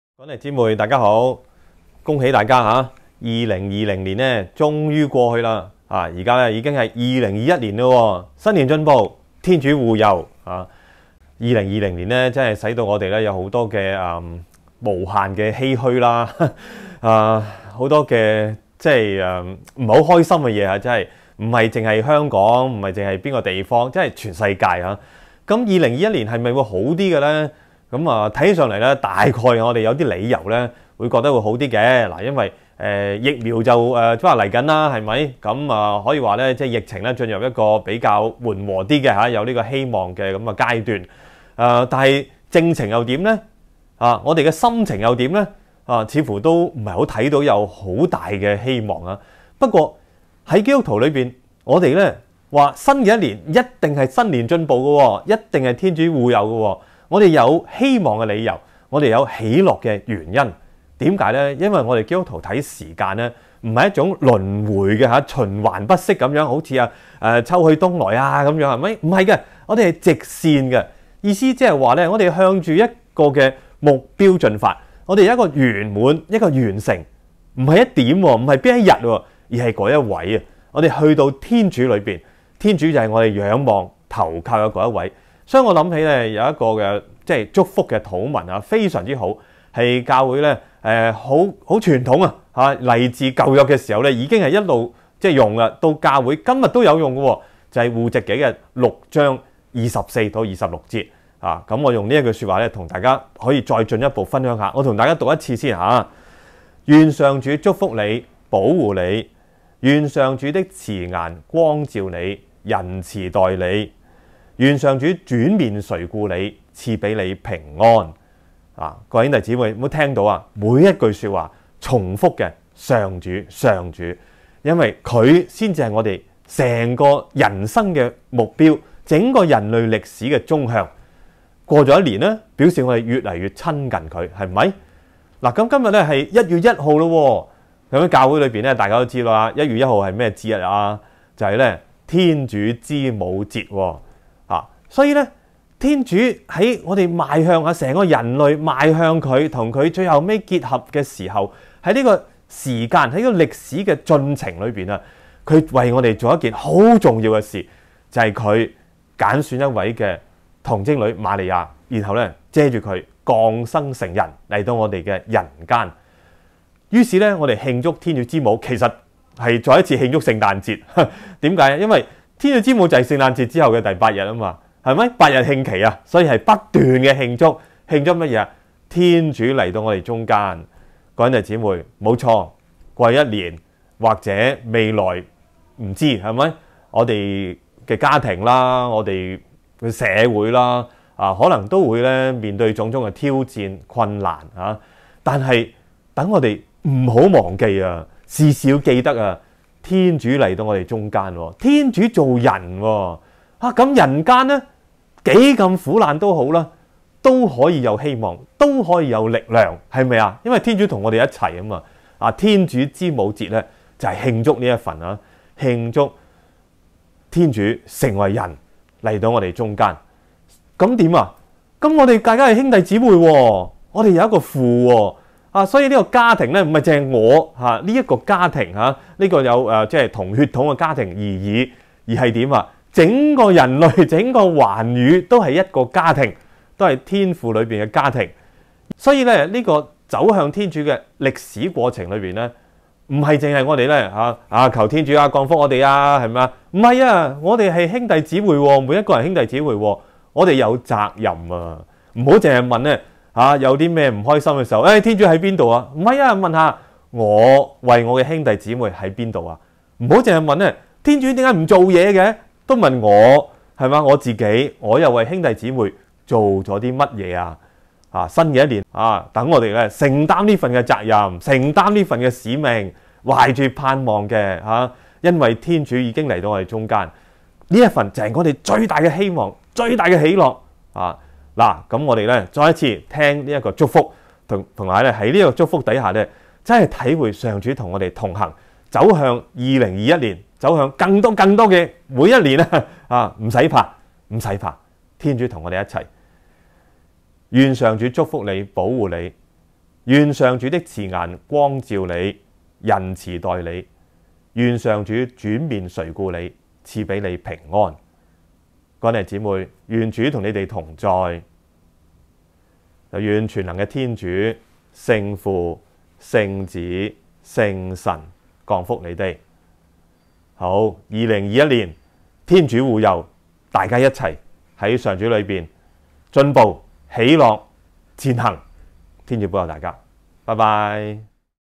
夏主教的2021年首個祝福！
"天主，為了2020年，感謝袮!
感謝袮讓我們人類
在世紀病毒面前，有機會學習謙抑自下;
在無力招架的疫境之中，體驗到世界原是一家;
在擔心社交距離的同時，明瞭彼此唇齒相依。
在這一年將盡的時刻，
但願我們懂得珍惜這份史無前例的恩賜，
發現其中的美善，
如同在要拆卸的廢址裡，能夠發現百年古蹟一樣。
懷著信心，迎向2021年，
因為祢仍然在引領我們、祝福我們。
袮就是那位與我們同在、同行的天主。
袮永生永王。阿孟。" - 夏主教撰寫的2020年回顧祈禱文